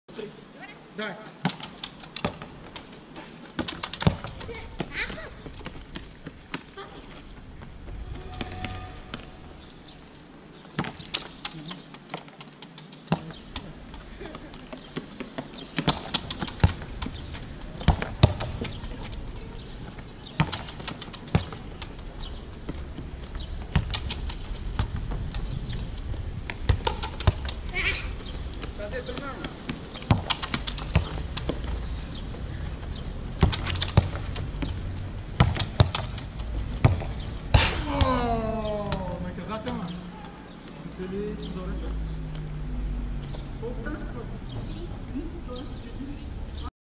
Стеничка (игра) ⚽️ е българска детска игра, която се играе с топка, най-често фут...
НАЧАЛНО УЧИЛИЩЕ “ЯНЕ САНДАНСКИ”
Вечните игри
Стеничката е българска детска игра, която се играе с топка, най-често футболна, на стена, ограда или друго подходящо преграждение. За играта на стеничка са нужни поне двама участника. Целта на играта е единият от участниците така да ритне топката в стената, че следващият след него да не може да я пресрещне навреме и от своя страна да я ритне в стената. Подобни игра съществува и в тениса, а същата концепция е използвана и в скуоша. Участниците се подреждат по ред и играят. Първия не успял с удар на топката да удари стената.